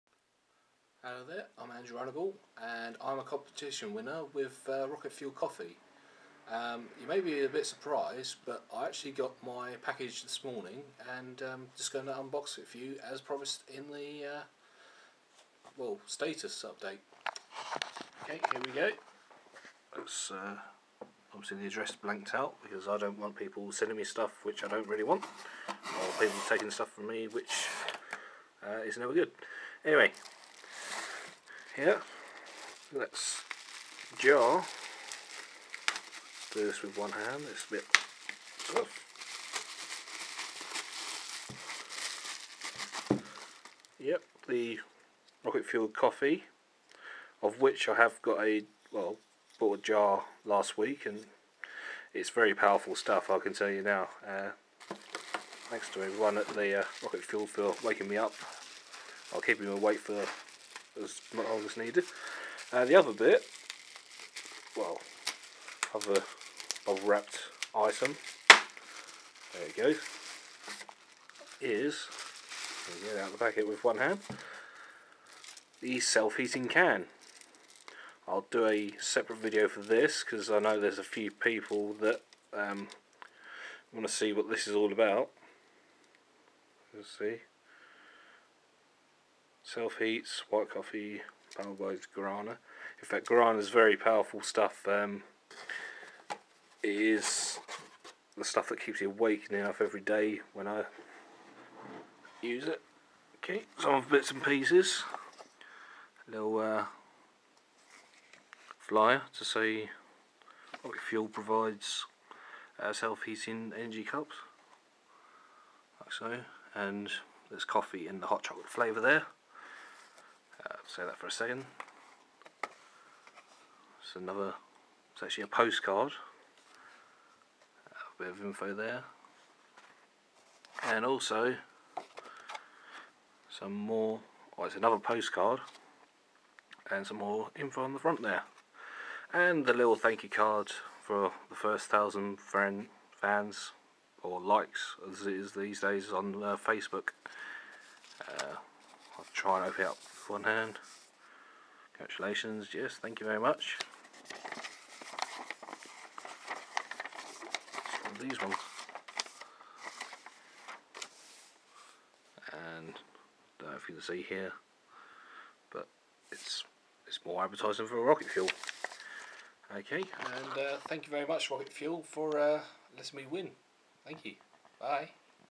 Rocket Fuel Coffee Competition Winnings
As promised, here is my unboxing video of the Rocket Fuel Coffee Freeb's pack.

Thank you to everyone at Rocket Fuel Coffee, keep up the good work.